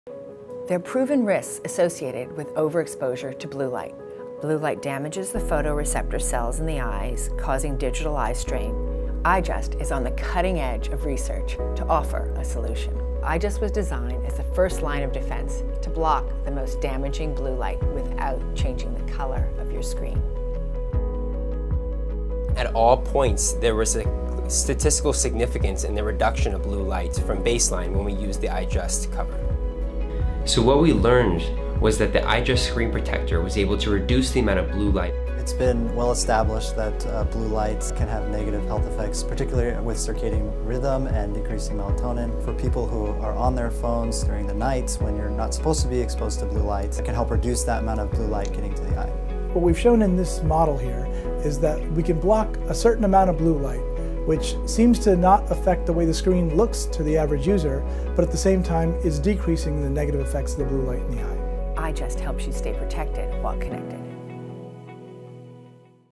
EyeJust Ophthalmologist Approved
EyeJust is a blue light blocking screen protector that does not change the look or feel of the screen. EyeJust was tested at the Gavin Herbert Eye Institute at UC Irvine. Studies proved that EyeJust reduces the amount of blue light which has negative impacts on overall health.